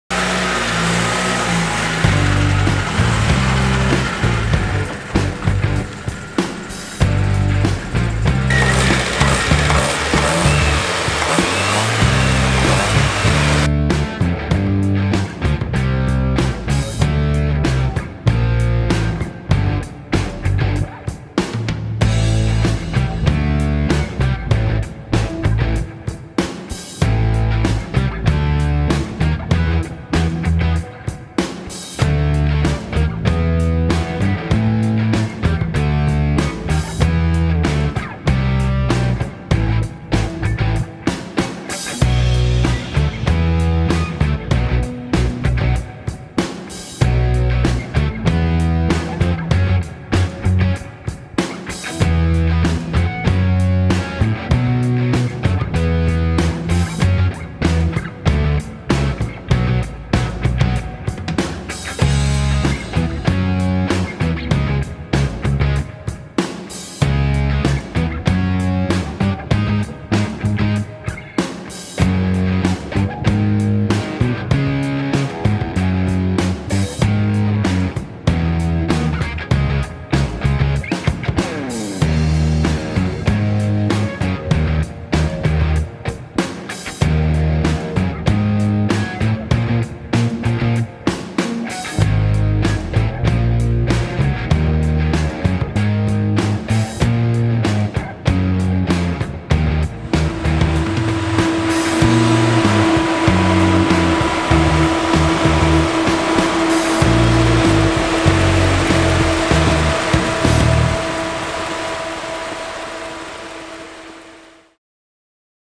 Land Rover series 3 SWB Pick up 1971 korte impressie
Deze Land Rover series 3 SWB Pick up uit 1971 is geheel opnieuw opgebouwd op het originele frame die is opgeknapt en verzonken. Een mooie prestatie van eigenaar Richard Bakker